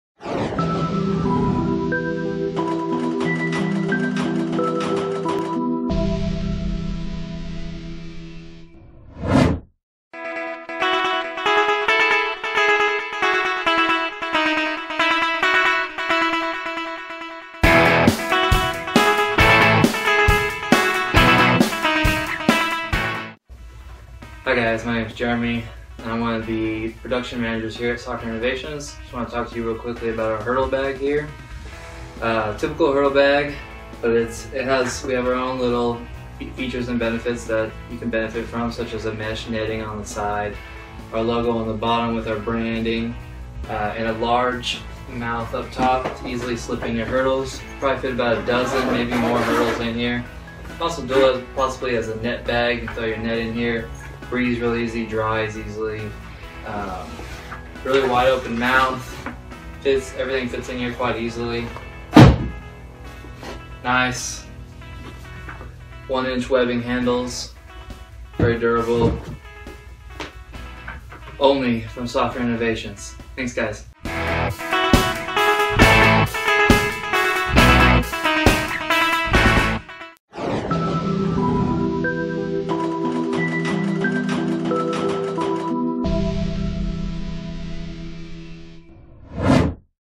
Soccer Hurdle Bag
Tote your soccer hurdles, speed and agility tools and soccer goal nets easily with our easy to load wide opening carry bag. Zipper extends along both sides to allow every soccer coach to easily insert your soccer equipment into our hurdle and net bag.